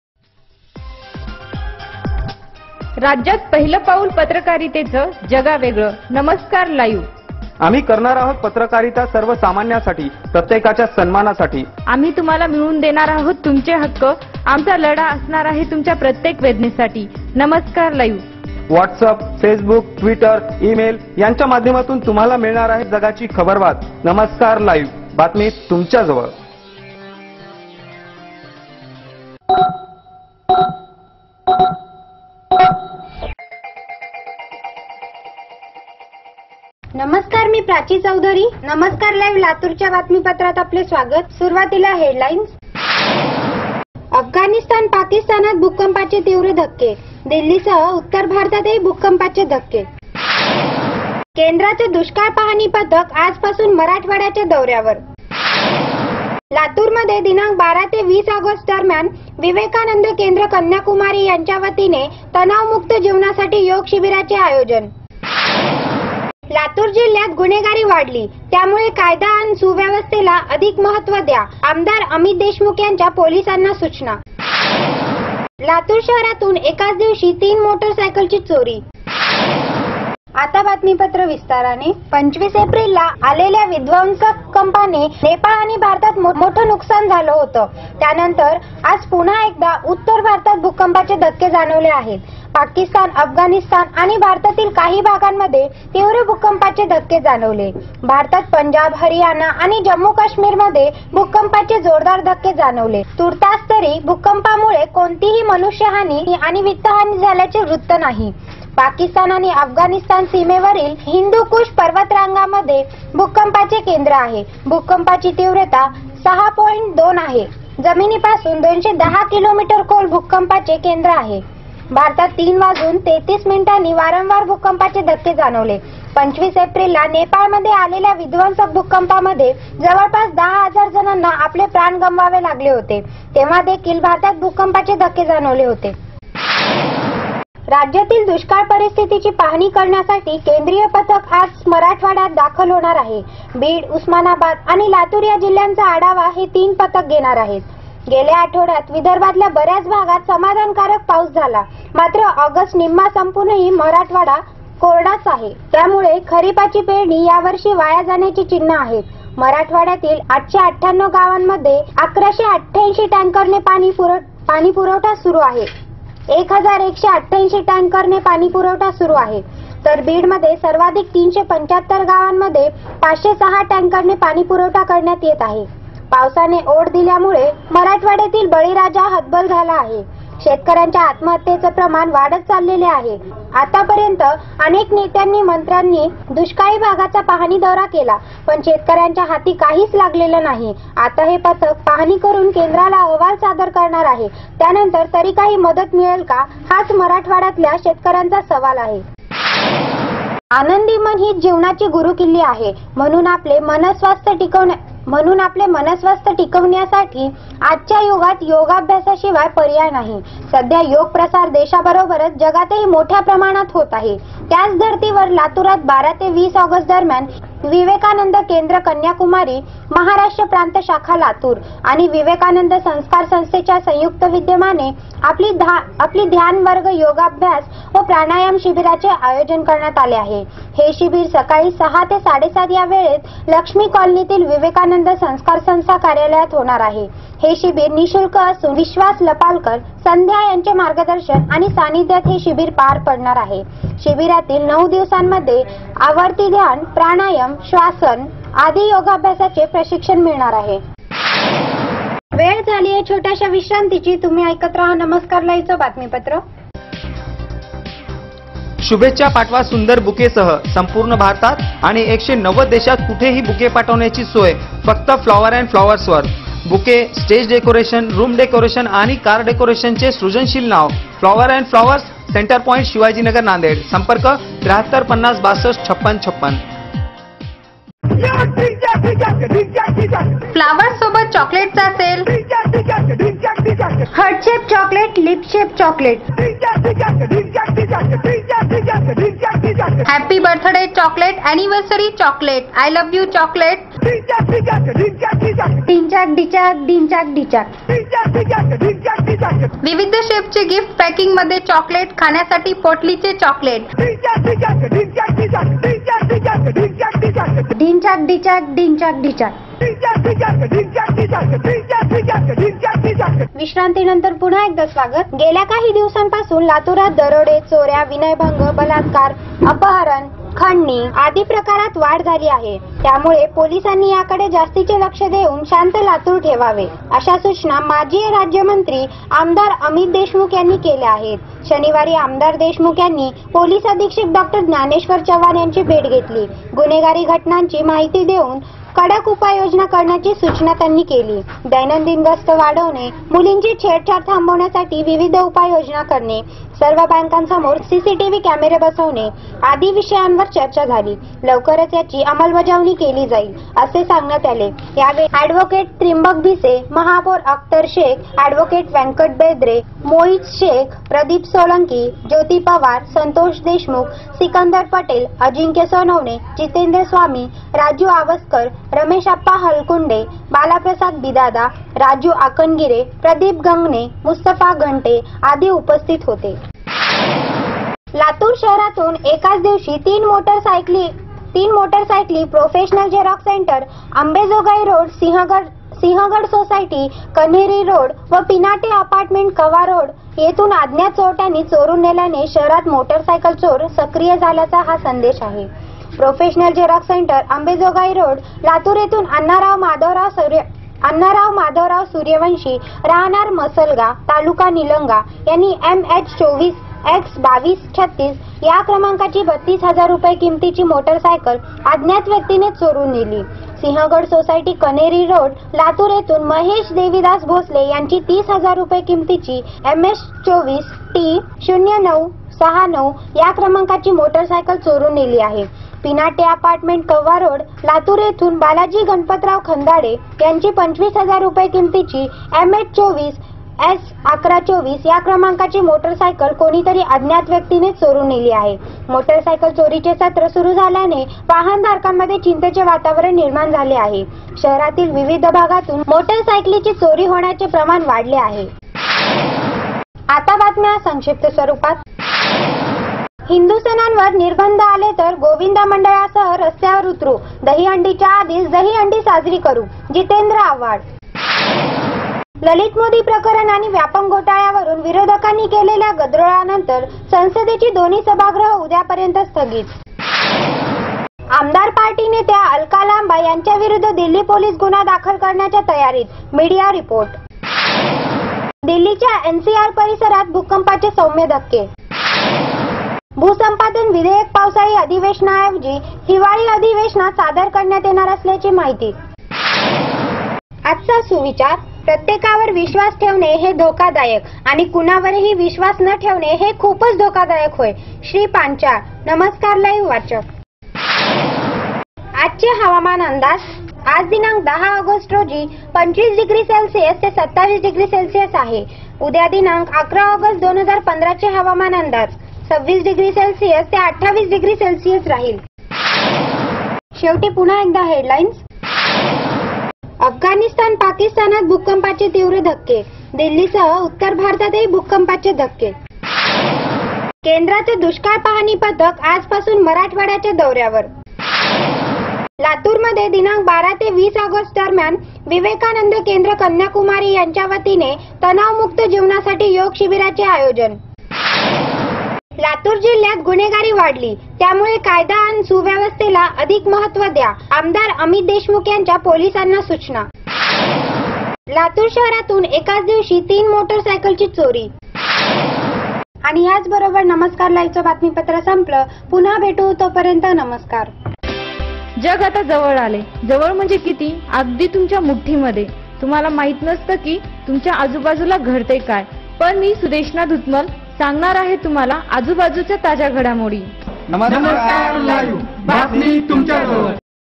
Namaskar Live 10-08-2015 Latur Audio News
नमस्कार लाईव्ह १०-०८-२०१५ लातूरचे बातमीपत्र

१- अफगानिस्तान, पाकिस्तानात भूकंपाचे तीव्र धक्के, दिल्लीसह उत्तर भारतातही भूकंपाचे धक्के
२- केंद्रचं दुष्काळ पाहणी पथक आजपासून मराठवाड्याच्या दौऱ्यावर
३- लातूरमध्ये दि. १२ ते २० ऑगस्ट दरम्यान विवेकानंद केंद्र कन्याकुमारी यांच्या वतीने तणावमुक्त जीवनासाठी योग शिबिराचे आयोजन
४- लातूर जिल्ह्यात गुन्हेगारी वाढली त्यामुळे कायदा अन सुरक्षा व्यवस्थेला अधिक महत्व द्या, आ. अमित देशमुख यांची पोलिसांना सूचना
५- लातूर शहरातून एकाच दिवशी तीन मोटरसायकलची चोरी
संक्षिप्त
1- हिंदू सणांवर निर्बंध आले तर, गोविंदा मंडळांसह रस्त्यावर उतरू, दहिहंडीच्या आधीच दहिहंडी साजरी करू - जितेंद्र आव्हाड
2-ललित मोदी प्रकरण आणि व्यापमं घोटाळ्यावरून विरोधकांनी केलेल्या गदारोळानंतर संसदेची दोन्ही सभागृह (लोकसभा-राज्यसभा) उद्यापर्यंत स्थगित
3-आप नेत्या अलका लांबा यांच्याविरुद्ध दिल्ली पोलीस गुन्हा दाखल करण्याच्या तयारीतः मीडिया रिपोर्ट
4-दिल्लीच्या एनसीआर परिसरात भूकंपाचे सौम्य धक्के
५- भूसंपादन विधेयक पावसाळी अधिवेशानाऐवजी हिवाळी अधिवेशनात सदर करण्यात येणार असल्याची माहिती
सुविचार
प्रत्येकावर विश्वास ठेवणे हे धोकादायक आणि कुणावरही विश्वास न ठेवणे, हे खूपच धोकादायक होय
( श्री पांचाळ, नमस्कार लाईव्ह वाचक }
हवामान अंदाज
आज २५ºC / २७ºC आहे
उद्या दि. ११ ऑगस्ट २०१५चे हवामान अंदाज
२६ºC / २८ºC राहील